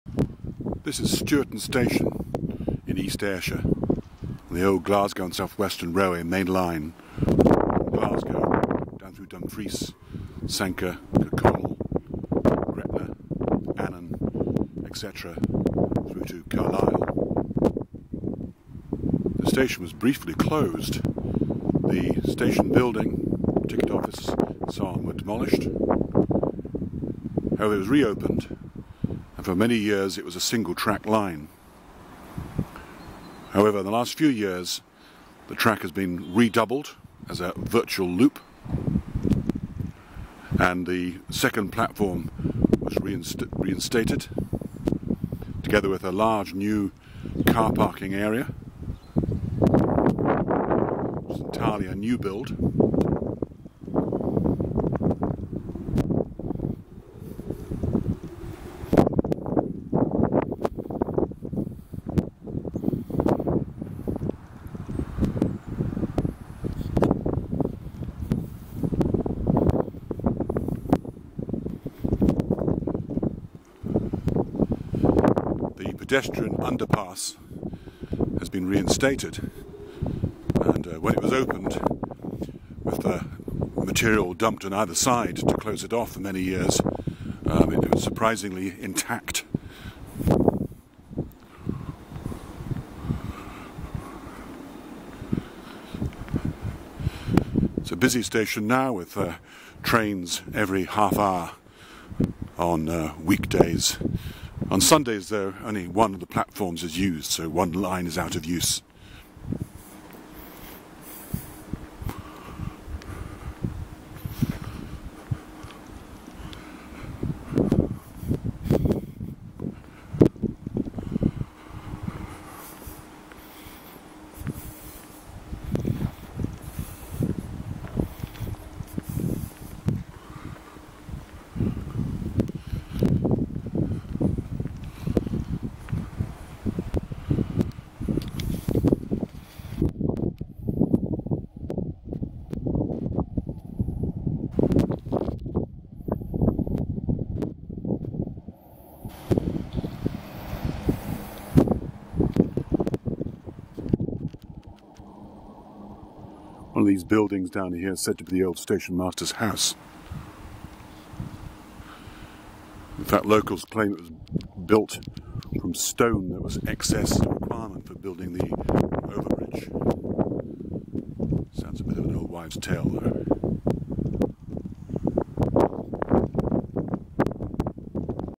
Stewarton Railway Station, East Ayrshire, Scotland
The station was opened on 27 March 1871 by the Glasgow, Barrhead and Kilmarnock Joint Railway. The station closed on 7 November 1966, however it reopened on 5 June 1967. In April 2007 a 'Ticket Issuing Machine' was installed in recognition of the relatively high passenger levels.

Operations to reinstate a second line between Lochridge Junction near Stewarton and Lugton started in 2008. With the work completed in September 2009, the second platform was reinstated, the pedestrian underpass re-opened and full disabled access provided. An overspill car park opened on 31 January 2012, accessed from platform 2.

Stewarton opened for goods traffic on 23 March 1871 and closed on 5 October 1964. It handled general goods as well as livestock and horseboxes.